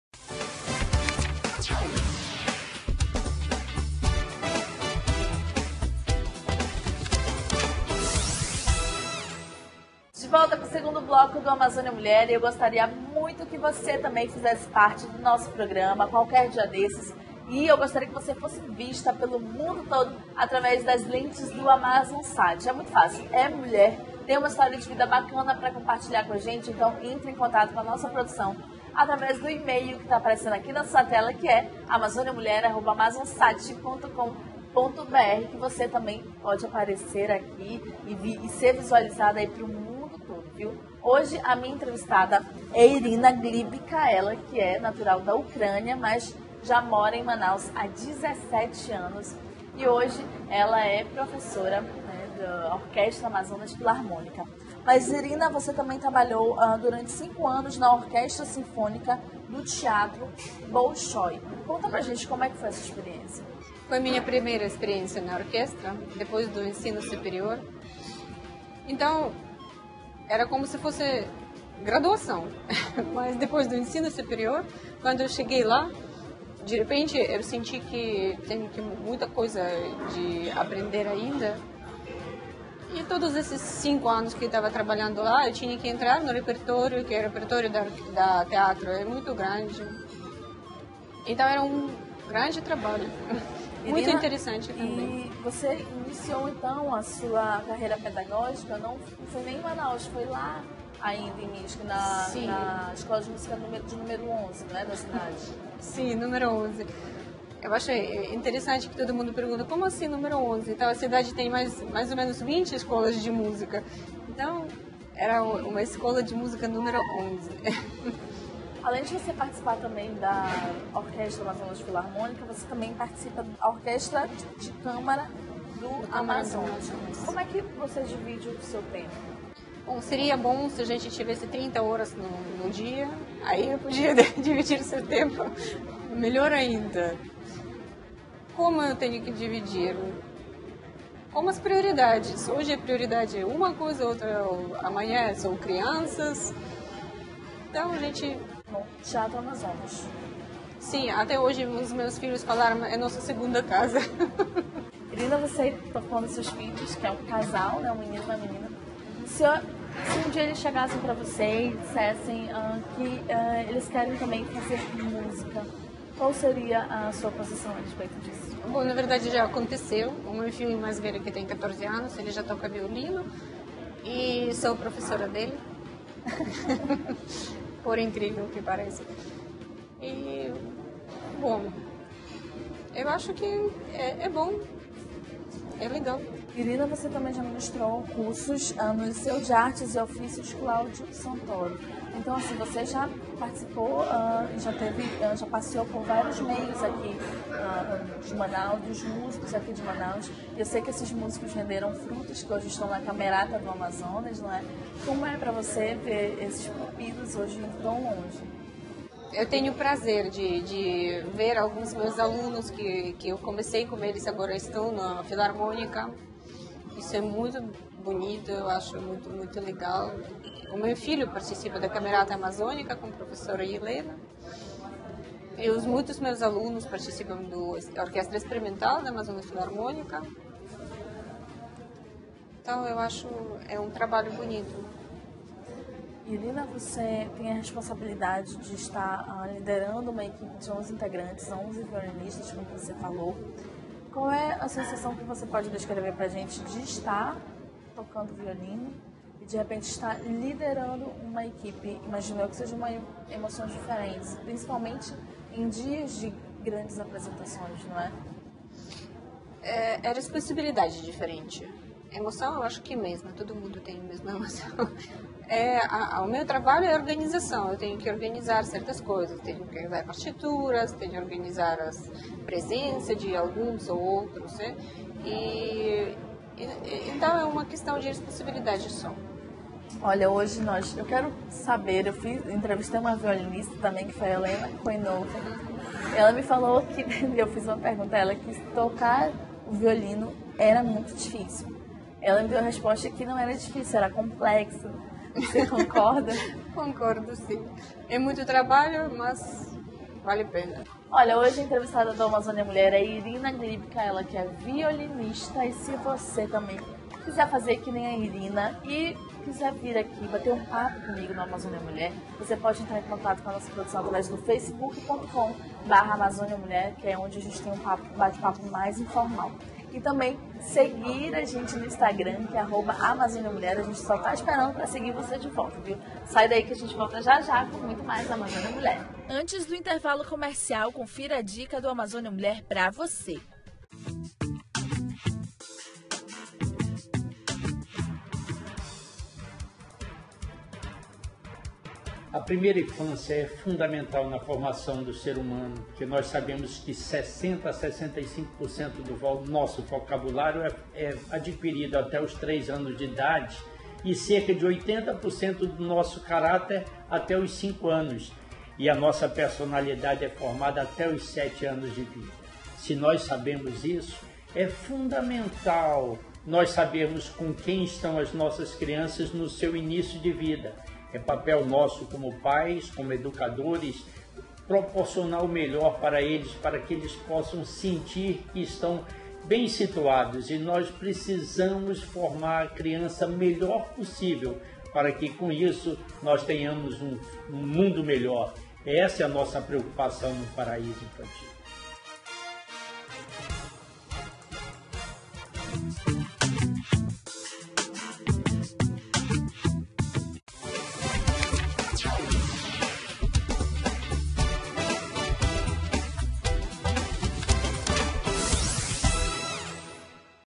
Amazônia Mulher | Irina Glibka | Parte 2
A Violinista Irina Glibka, fala da sua trajetória de vida e a vinda para Manaus.